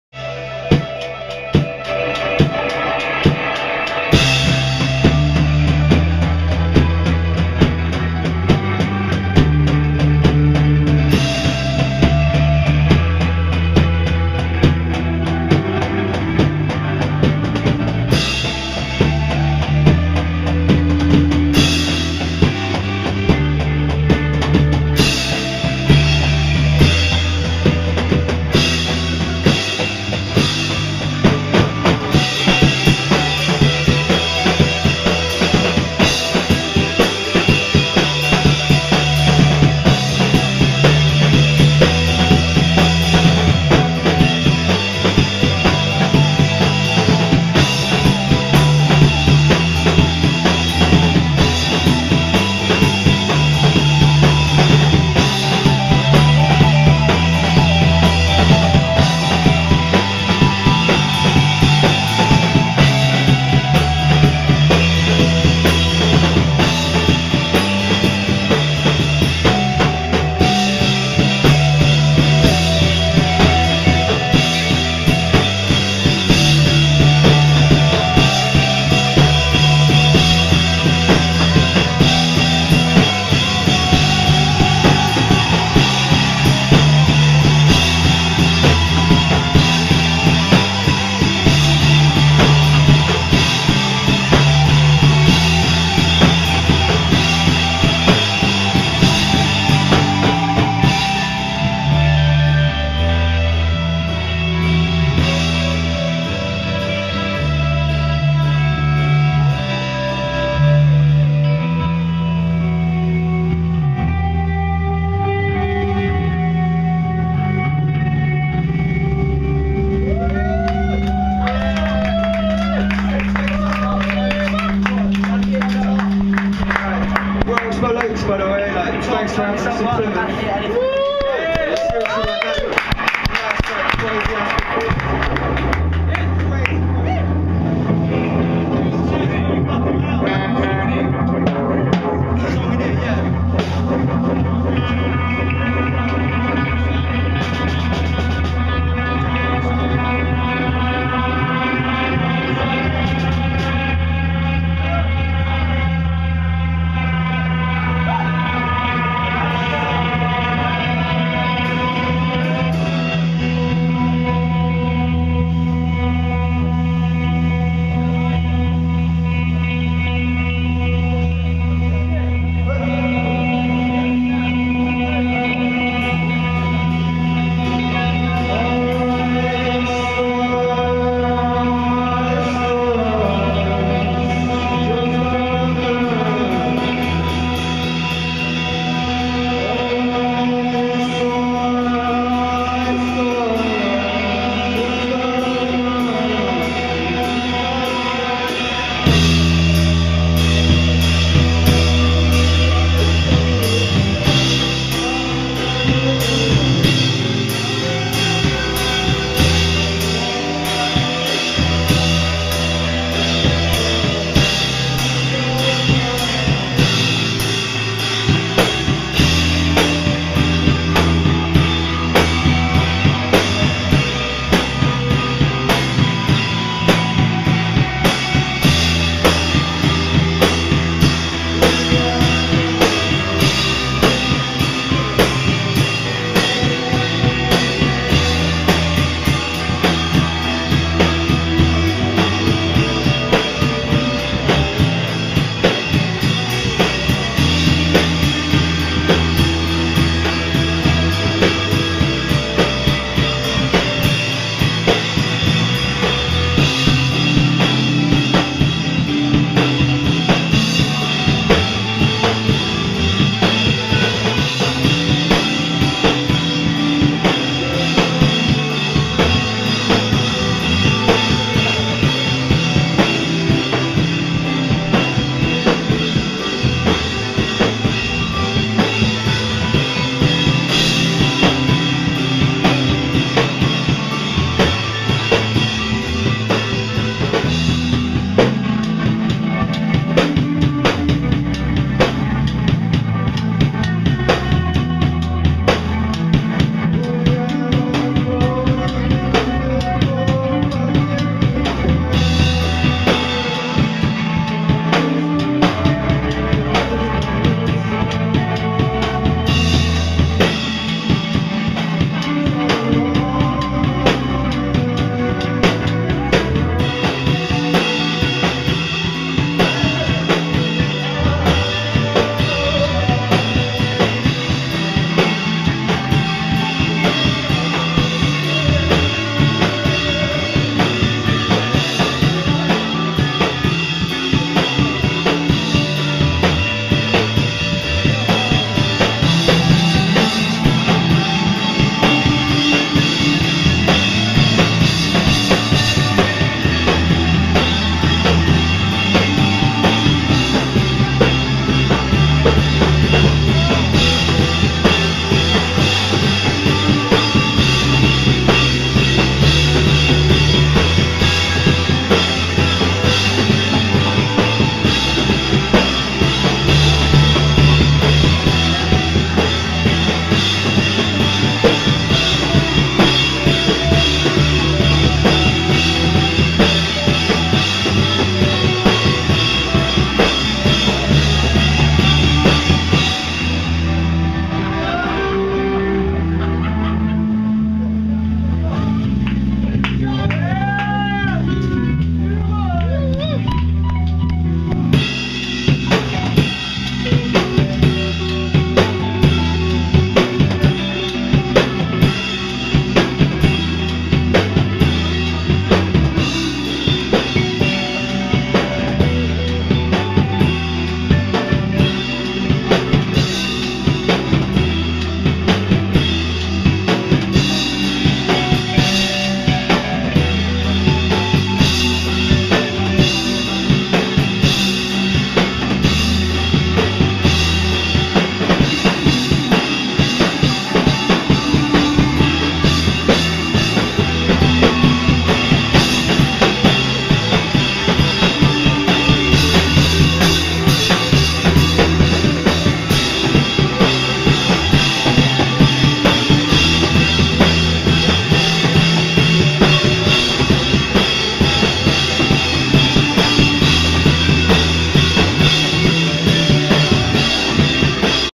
Oxbow Lakes live
Oxbow Lakes playing at my house show in Plymouth.